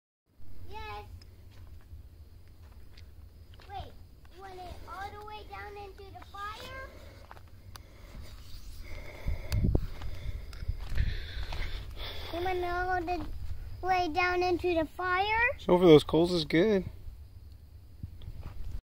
Video of LBJ National Grasslands Forest Road 904 Dispersed Camping, TX from Christopher L.
kid roasting his first camp hot dog

▶︎ simple beauty
First trip here last week, early April. Remote, quiet, simple beauty. Easy access, cool area. Worth checking out for a low profile weekender or hike.